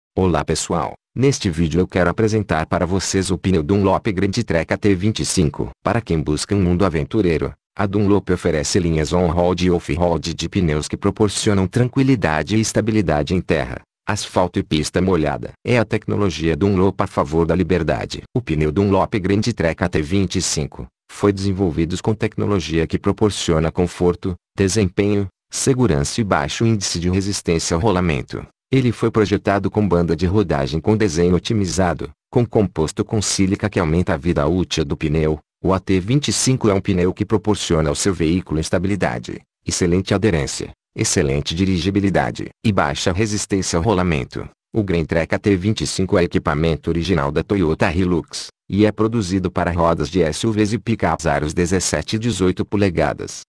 PNEU DUNLOP GRANDTREK AT25
👍 COMPRE NO MAGAZINEVOLGRAN EM 10 X SEM JUROS  PARCEIRO MAGALU

DUNLOP  GRANDTREK AT25
Pneus desenvolvidos com tecnologia que proporciona conforto, desempenho, segurança e baixo índice de resistência ao rolamento.

CARACTERÍSTICAS
Banda de rodagem com desenho otimizado;
Composto com sílica.
VANTAGENS
Estabilidade;
Excelente aderência;
Excelente dirigibilidade.
baixa resistência ao rolamento

(Equipamento Original da Toyota Hilux)

Medidas por Aro
17"
265/65R17 112S
18"
265/60R18 110H